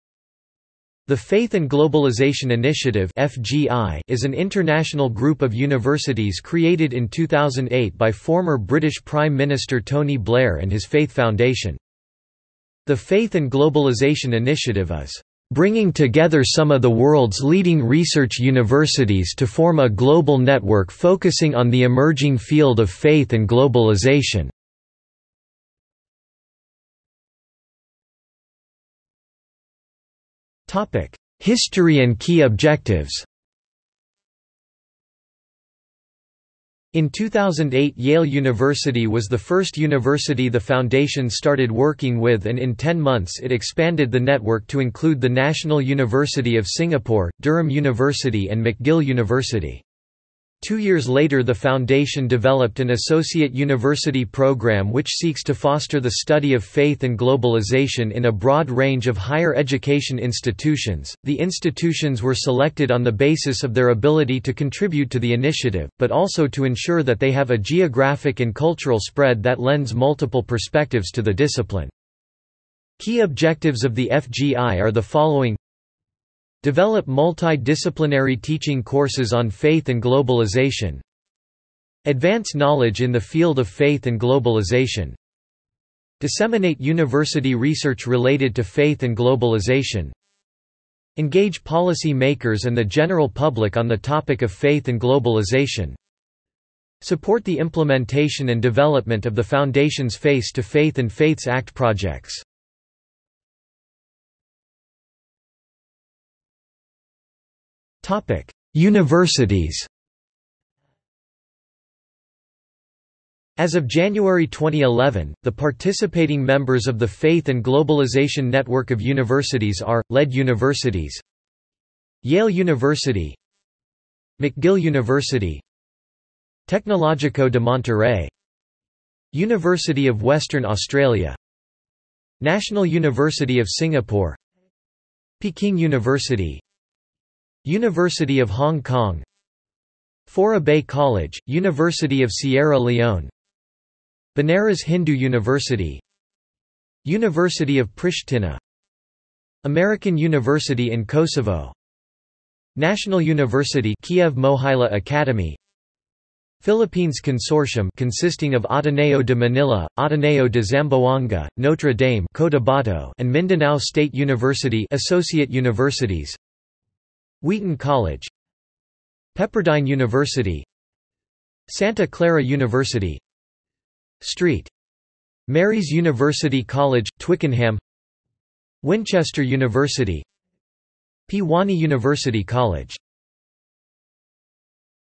Faith and Globalisation Network of Universities | Wikipedia audio article
00:00:27 1 History and key objectives
00:01:44 2 Universities

- Socrates

SUMMARY
The Faith and Globalisation Initiative (FGI) is an international group of universities created in 2008 by former British prime minister Tony Blair and his Faith Foundation.
The Faith and Globalisation Initiative is "bringing together some of the world’s leading research Universities to form a global network focusing on the emerging field of faith and globalisation".